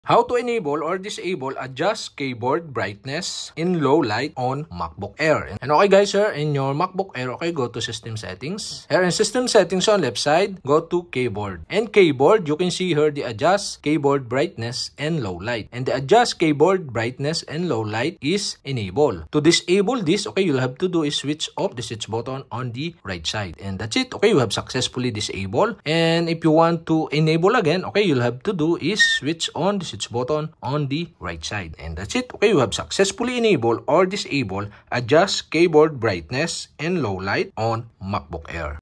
How to Enable or Disable Adjust Keyboard Brightness in Low Light on MACBOOK
In this tutorial i will teach you on how to enable or disable adjust keyboard brightness in low light on MACBOOK AIR.